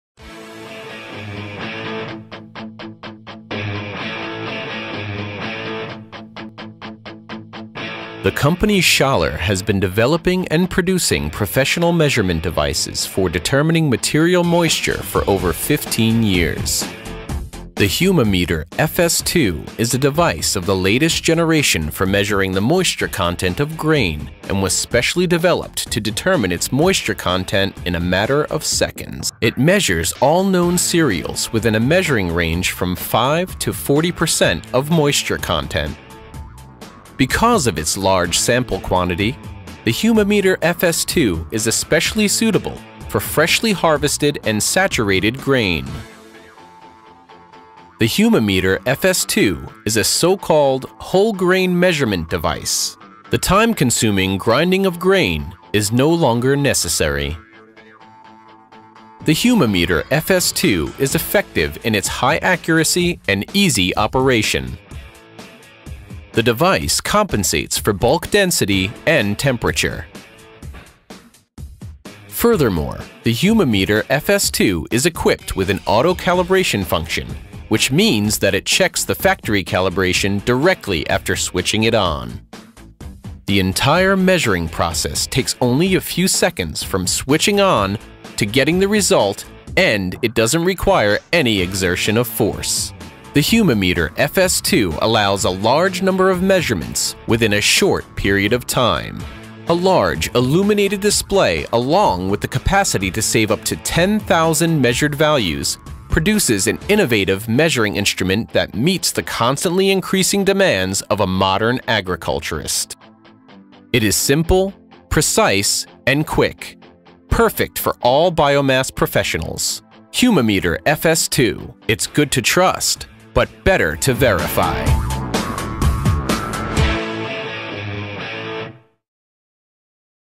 humimeter FS2, FS3, FS4 Grain moisture meter
Whole grain meter with 300g sample weight and sample temperature measurement.
For use in farming, trade and animal feed industries.
Grain moisture meter for maize, rye, Triticale, wheat, spelt, barley, oats, rape, pumpkin seeds,
peas, soybeans, beetle beans, horse beans ,Jatropha and sunflower seeds
Whole grain meter for all grain types

Quality check by measurement within seconds without prior treatment of samples